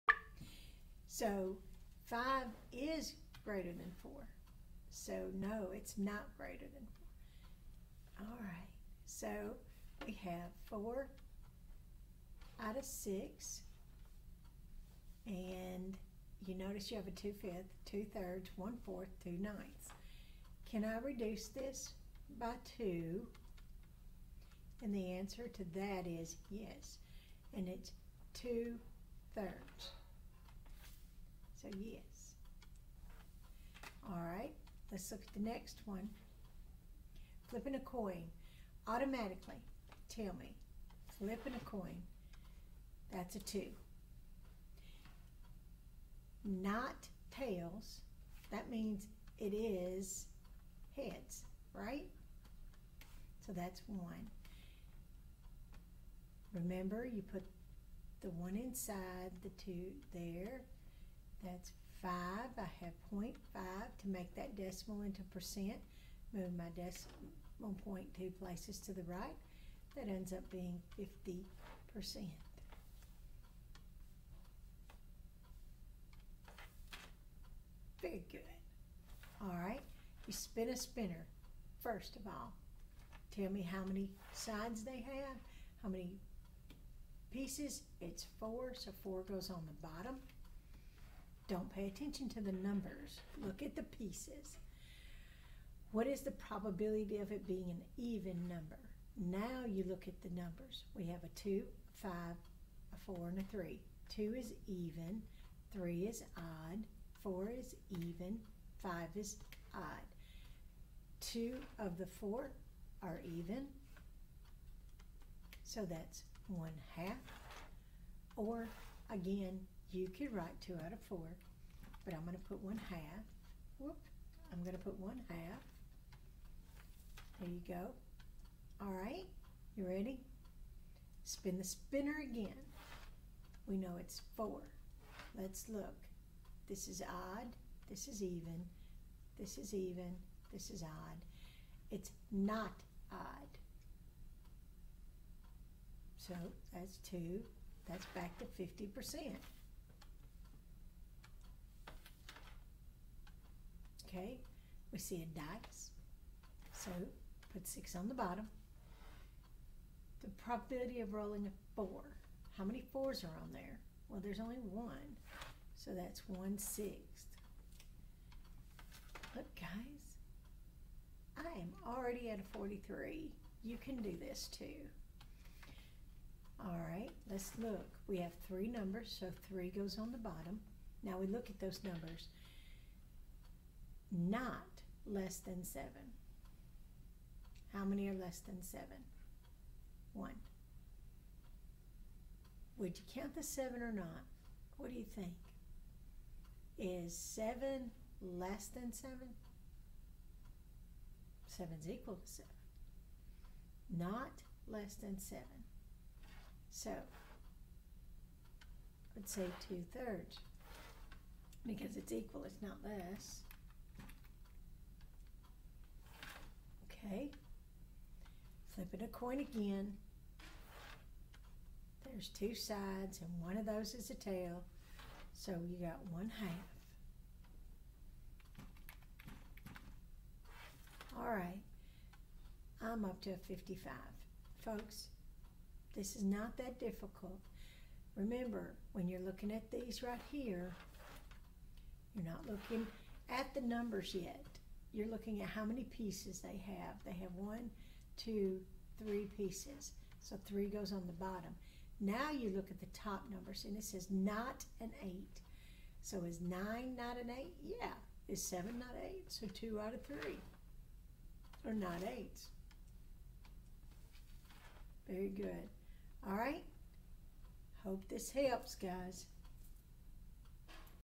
7th Grade Math IXL DD2 Probability of simple events and opposite events
IXL 7th grade Math DD2 solving probability of simple events and opposite events